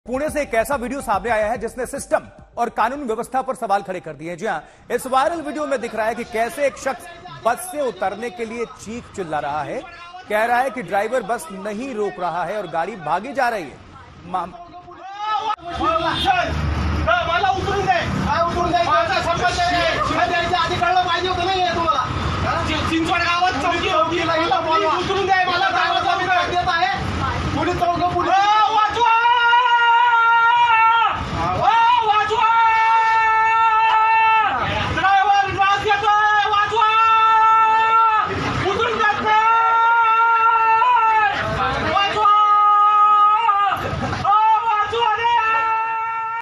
मनचाही जगह उतरने के लिए यात्री का हंगामा | Pune Bus Video Viral | Trending News | Latest
Pune News: एक वायरल वीडियो में बस सवार शख्स चिल्लाते दिख रहा है कि मुझे बचा लो. ड्राइवर मुझे किडनैप कर रहा है. इस दौरान यात्री ने बस ड्राइवर से बहस और गाली गलौज भी की. बस में मौजूद किसी अन्य यात्री ने इसका वीडियो बना लिया और वायरल कर दिया. पूरा मामला जानकर आप भी हैरान रह जाएंगे...
Pune News: In a viral video, a man in a bus is seen shouting to save me. The driver is kidnapping me. During this, the passenger also argued and abused the bus driver. Another passenger present in the bus made a video of it and made it viral. You will also be surprised to know the whole matter...